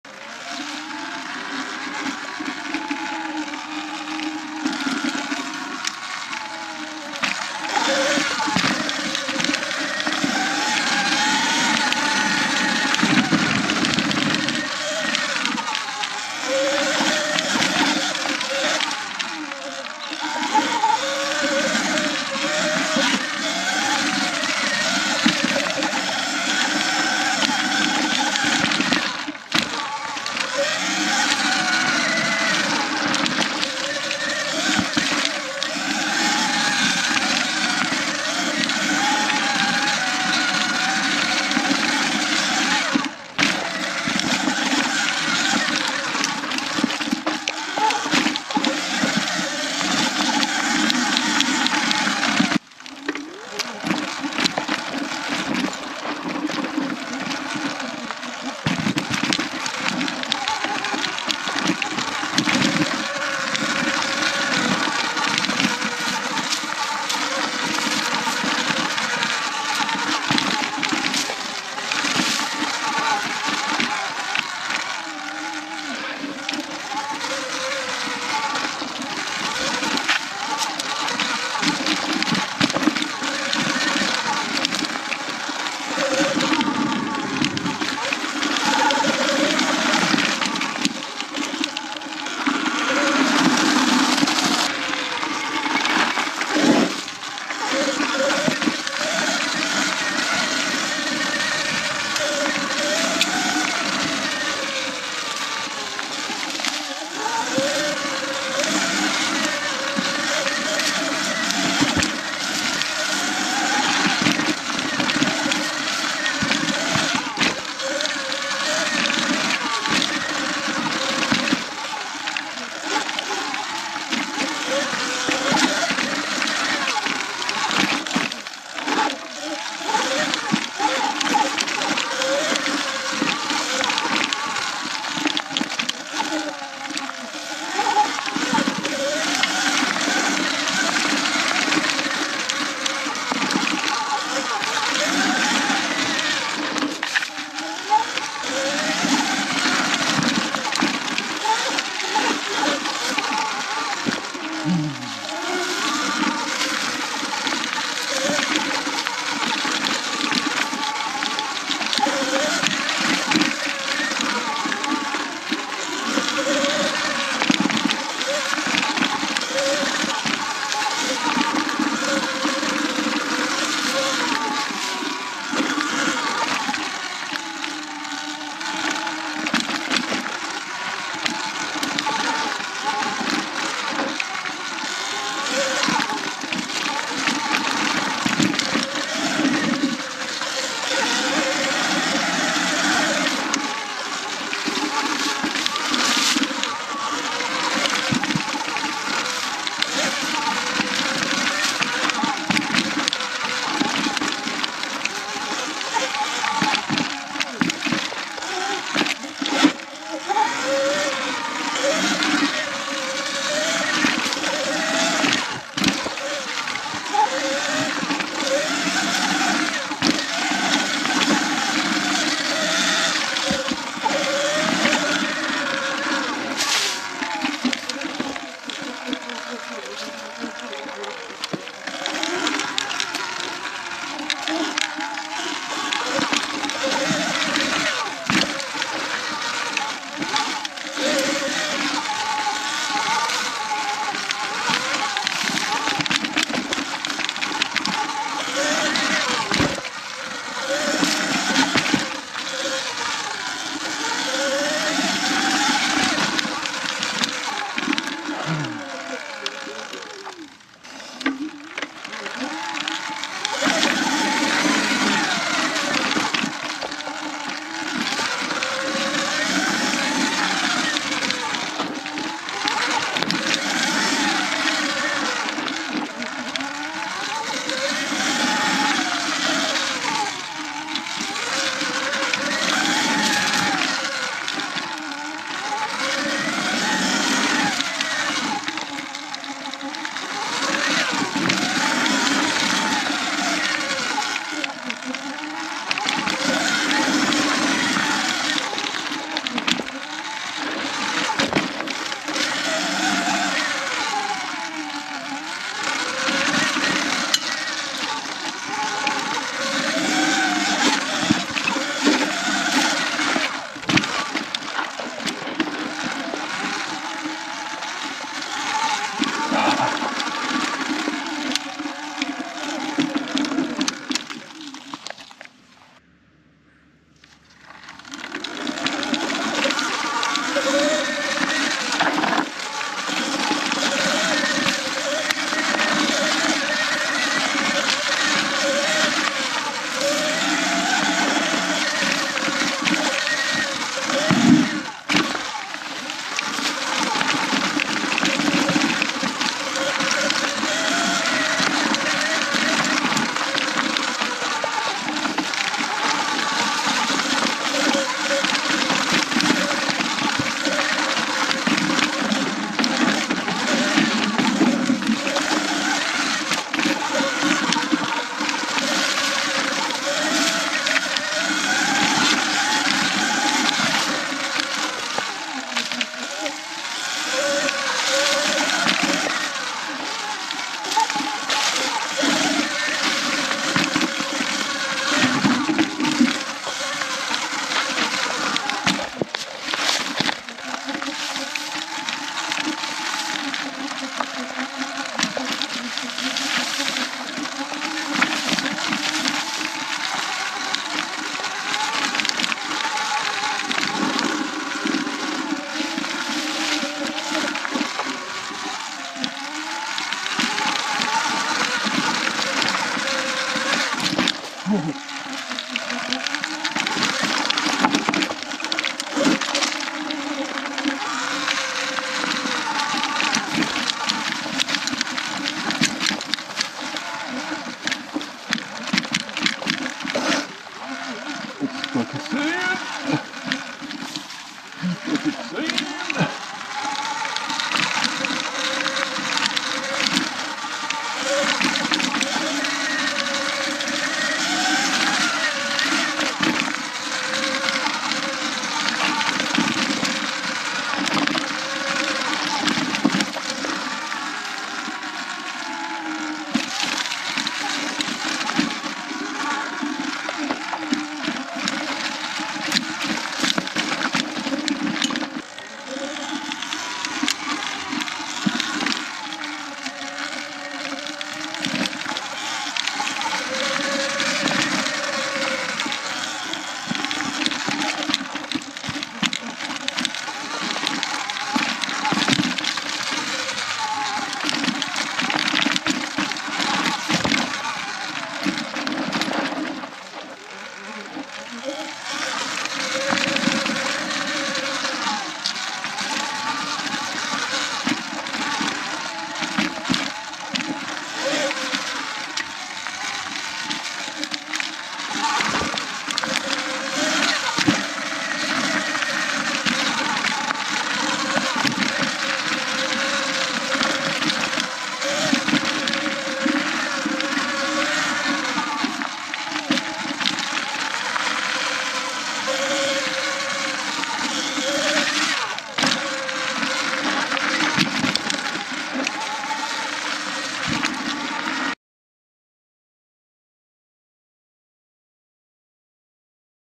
Sur Ron X - Another Day At the Gulch
Working on the courage to hit the jumps on Orion, got one today!